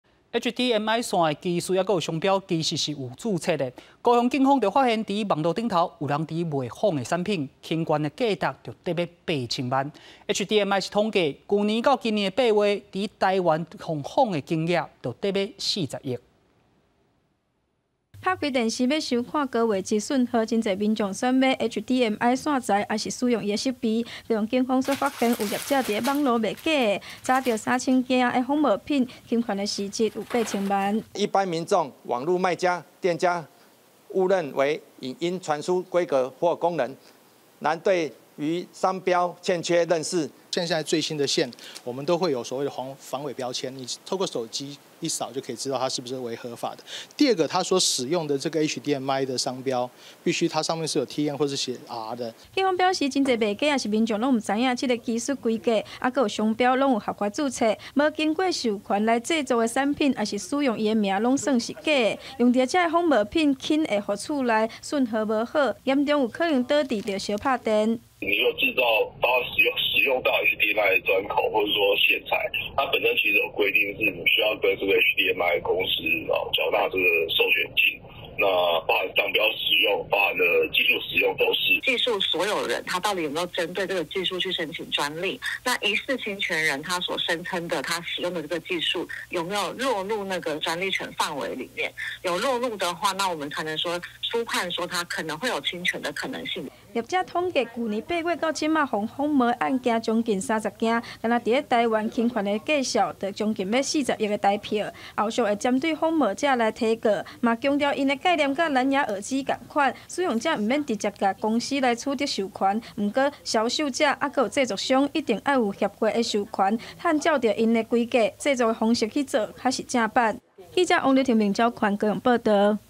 警查獲網售未授權HDMI仿品 侵權市值近8千萬｜20231129 公視中晝新聞
HDMI線是不少民眾生活中必備3C線材，但有很多人誤以為HDMI只是影音傳輸介面，卻不知道它的技術規格跟商標都已經註冊，高雄警方就查獲網路賣家賣仿冒產品侵權市值將近8000萬元，HDMI統計，光在台灣從去（2022）年到今年8月被仿冒金額已經將近40億元，將會針對這些不法行為提告。